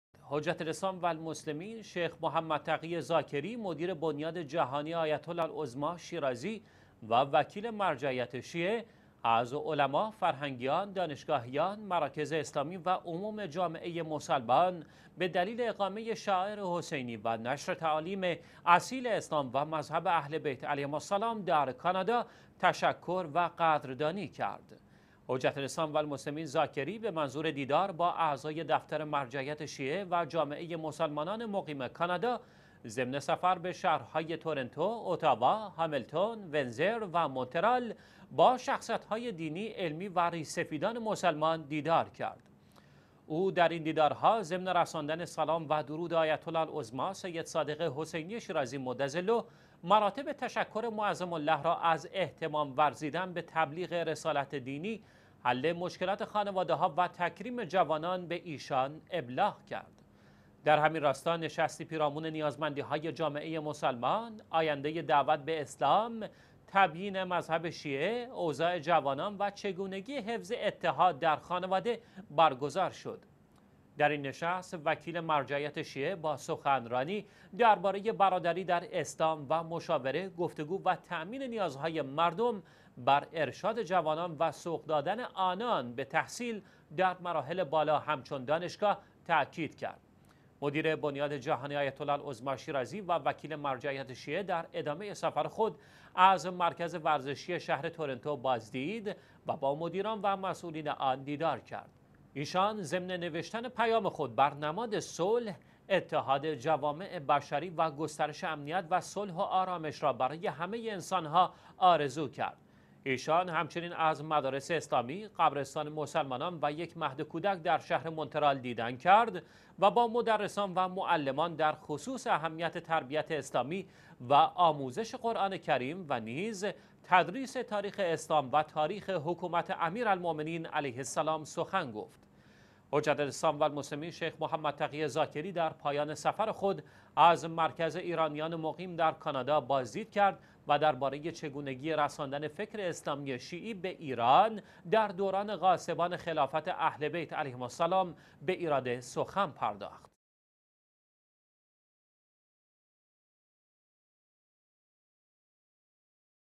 قدردانی از تلاش های علما و فرهنگیان در نشر اسلام اصیل در کانادا
حجت الاسلام والمسلمین «شیخ محمد تقی ذاکری» مدیر «بنیاد_جهانی_آیت_الله_العظمی_شیرازی» و وکیل مرجعیت شیعه، از علما، فرهنگیان، دانشگاهیان، مراکز اسلامی و عموم جامعه مسلمان، به دلیل اقامه شعائر حسینی و نشر تعالیم اصیل اسلام و مذهب اهل بیت علیهم السلام در «کانادا»، تشکر و قدردانی کرد.

حجت الاسلام والمسلمین ذاکری به منظور دیدار با اعضای دفتر مرجعیت شیعه و جامعه مسلمانان مقیم کانادا، ضمن سفر به شهر های «تورنتو»، «اوتاوا» ، «هاملتون»، «ونزر» و «مونترال»؛ با شخصیت های دینی، علمی و ریش سفیدان مسلمان دیدار کرد.

او در این دیدارها ضمن رساندن سلام و درود آیت الله العظمی سید صادق حسینی شیرازی مدظله، مراتب تشکر معظم له را از اهتمام ورزیدن به تبلیغ رسالت دینی، حل مشکلات خانواده ها و تکریم جوانان، به ایشان ابلاغ کرد.

در همین راستا نشستی پیرامون نیازمندی های جامعه مسلمان، آینده دعوت به اسلام، تبیین مذهب شیعه، اوضاع جوانان، و چگونگی حفظ اتحاد در خانواده برگزار شد.

در این نشست، وکیل مرجعیت شیعه با سخنرانی در باره ی برادری در اسلام، و مشاوره، گفتگو و تامین نیازهای مردم، بر ارشاد جوانان و سوق دادن آنان به تحصیل در مراحل بالا همچون دانشگاه، تاکید کرد.

مدیر بنیاد جهانی آیت الله العظمی شیرازی و وکیل مرجعیت شیعه، در ادامه سفر خود، از مرکز ورزشی شهر تورنتو بازدید، و با مدیران و مسئولین آن دیدار کرد.

ایشان، ضمن نوشتن پیام خود بر نماد صلح، اتحاد جوامع بشری و گسترش امنیت و صلح و آرامش را برای همه انسان ها آرزو کرد.

ایشان همچنین از مدارس اسلامی، قبرستان مسلمانان و یک مهد کودک در شهر مونترال دیدن کرد و با مدرسان و معلمان در خصوص اهمیت تربیت اسلامی و آموزش قرآن کریم و نیز تدریس تاریخ اسلام و تاریخ حکومت امیرالمومنین علیه السلام، سخن گفت.

حجت الاسلام والمسلمین شیخ محمد تقی ذاکری در پایان سفر خود، از مرکز ایرانیان مقیم در کانادا بازدید کرد و در باره ی چگونگی رساندن فکر اسلامی شیعی به ایران در دوران غاصبان خلافت اهل بیت علیهم السلام، به ایراد سخن پرداخت.